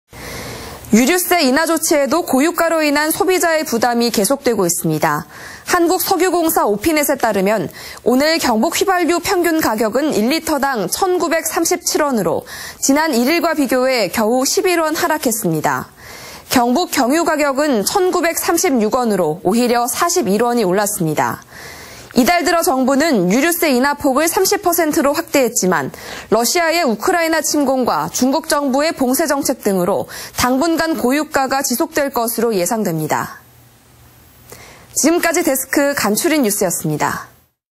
유류세 30% 인하에도 기름값 부담 여전 / 안동MBC
2022/05/11 13:53:28    작성자 : 윤소영

유류세 인하 조치에도,
고유가로 인한 부담은 계속되고 있습니다.
◀ＥＮＤ▶

한국석유공사 오피넷에 따르면,
오늘(11일) 경북 휘발유 평균 가격은 1리터당
1,937원으로 지난 1일과 비교해
겨우 11원 하락했습니다.

경북 경유 가격은 1,936원으로,
오히려 41원 올랐습니다.

이달 들어, 정부는 유류세 인하 폭을 30%로
확대했지만 러시아의 우크라이나 침공과
중국 정부의 봉쇄 정책 등으로,
당분간 고유가가 지속될 것으로 예상됩니다.